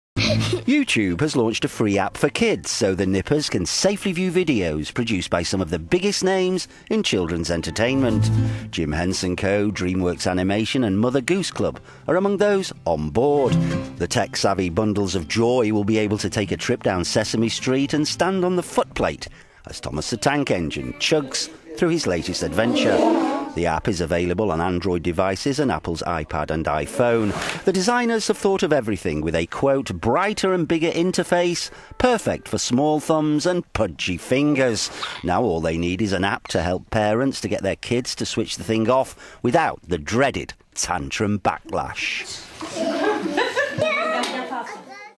YouTube Kids coming to a screen near you in your lounge, car or doctors waiting room
YouTube has launched a free app for kids so the nippers can safely view videos produced by some of the biggest names in children's entertainment.

Jim Henson Co, Dreamworks Animation and Mother Goose Club are among those on board.

The tech-savvy bundles of joy will be able to take a trip down 'Sesame Street" and stand on the footplate as 'Thomas the Tank Engine' chugs through his latest adventure.

The app is available on Android devices and Apple's IPad and IPhone.